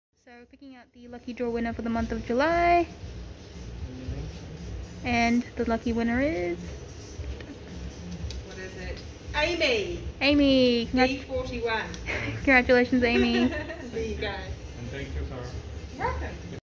Lucky draw july
In store July 2022 lucky draw announcement of winner, worth of $150 gift hamper.
Congratulations to @Amy🎊🎈🎉🎖🧧🥇
Special thanks to @Sarah for drawing the lucky dip winner.